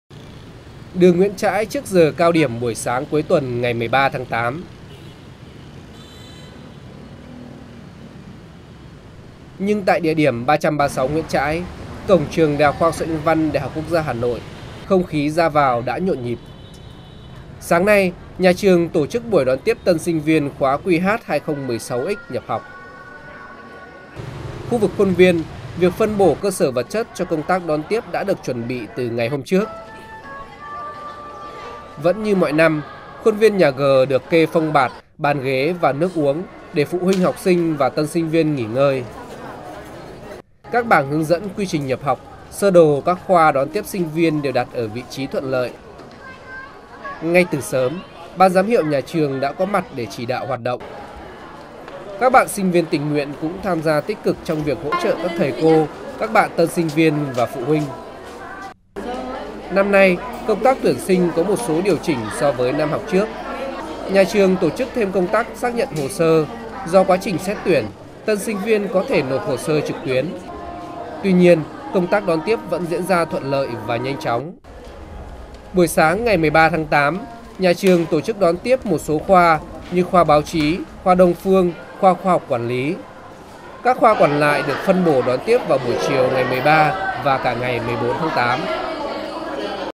Đón tân sinh viên khóa QH-2016-X nhập học
Trong hai ngày 13 và 14/8/2016, Trường ĐHKHXH&NV đã tổ chức đón tiếp hơn 1200 các bạn tân sinh viên khóa K61 nhà trường nhập học.